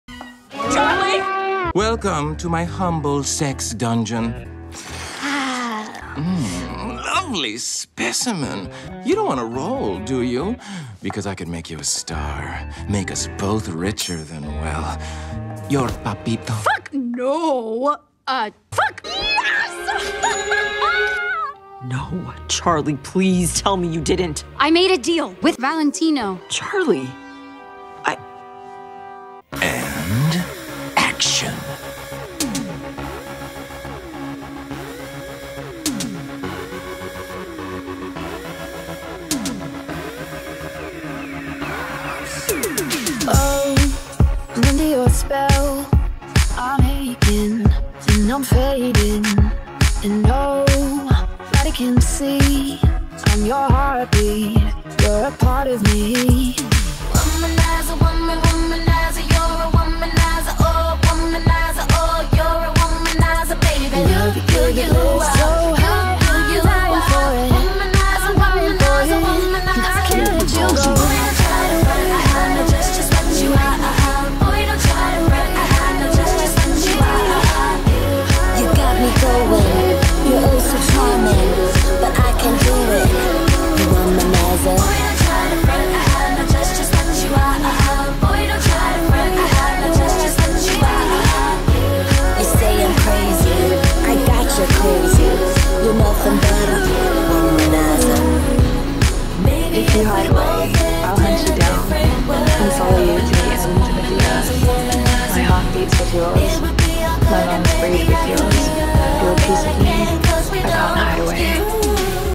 Charlie Accepts Valentino's Offer (Take 34)
Charlie takes up another offer as Valentino's charm is irresistible.

Thumbnail drawn by  @MelIsTato on Twitter.